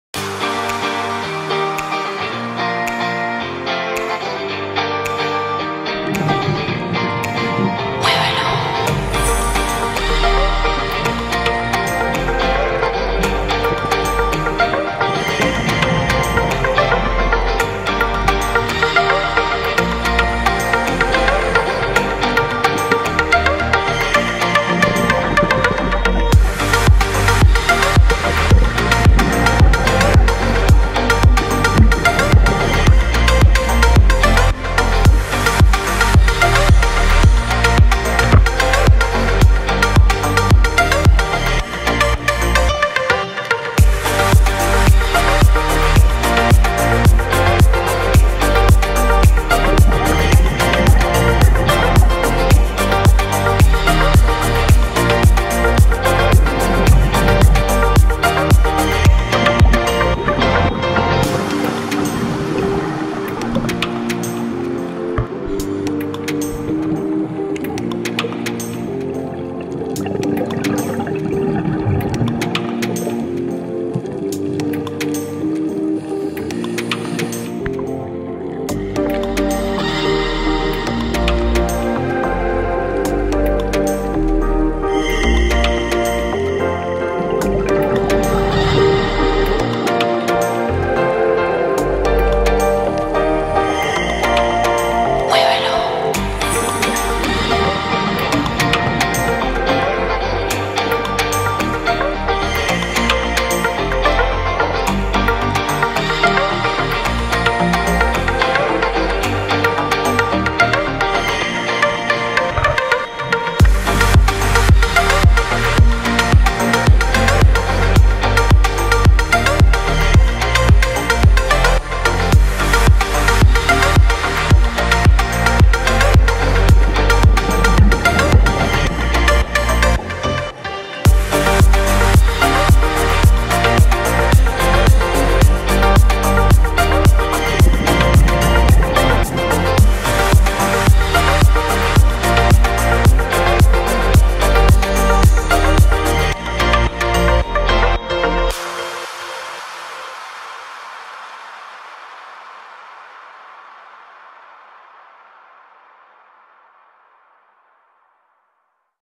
Diving with sharks in Bathala
Diving with grey reef sharks and white tip reef sharks, in Bathala, Ari Atoll, Maldives.